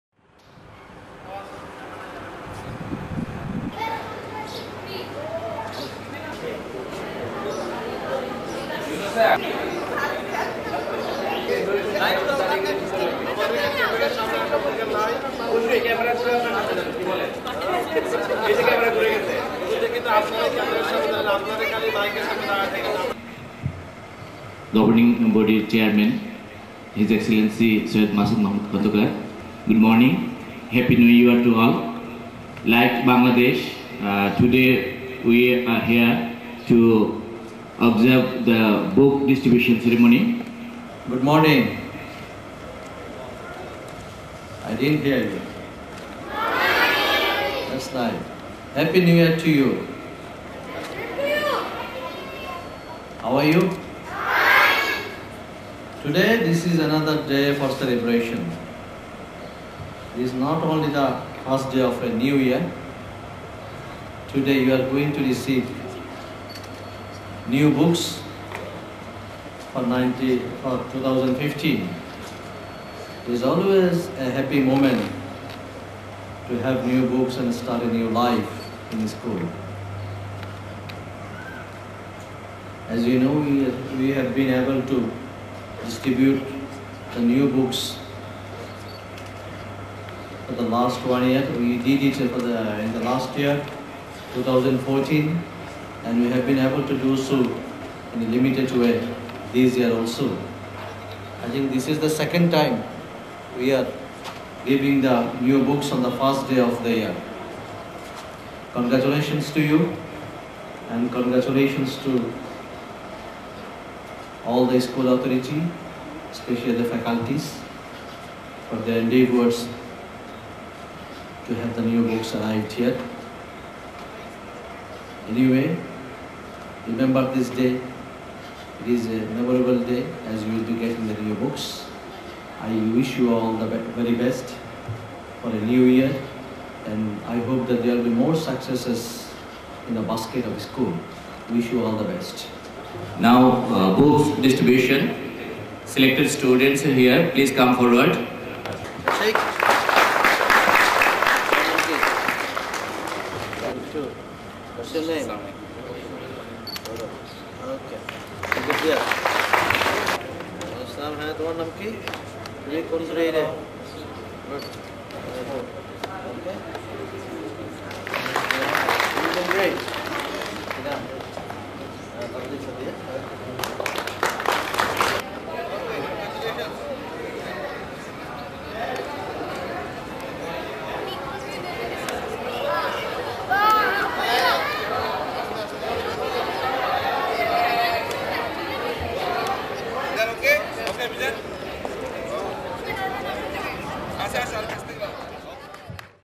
FREE TEXT BOOK DISTRIBUTION CERMONY 2015,DOHA QATAR
FREE TEXT BOOK DISTRIBUTION CEREMONY 2015 IN BANGLADESH MHM SCHOOL AND COLLEGE, DOHA ,QATAR.

LIKE LAST YEAR ,THIS YEAR ALSO FREE NEW BOOKS WERE DISTRIBUTED AMONG THE STUDENTS OF BANGLADESH MHM SCHOOL AND COLLEGE , IN QATAR ON 1-1-2015. HONORABLE AMBASSADOR OF BANGLADESH , HIS EXCELLENCY SYED MASUD MAHMUD KHONDOKAR INAUGURATED THE CEREMONY . PRINCIPAL MD. JASHIM UDDIN , MR. SHA ALAM , MEMBER OF THE GOVERNING BOARD AND TEACHERS OF THE INSTITUTION WERE PRESENT.  HONORABLE AMBASSADOR WISHED EVERYONE IN QATAR AND IN BANGLADESH A VERY  PROSPEROUS AND HAPPY NEW YEAR.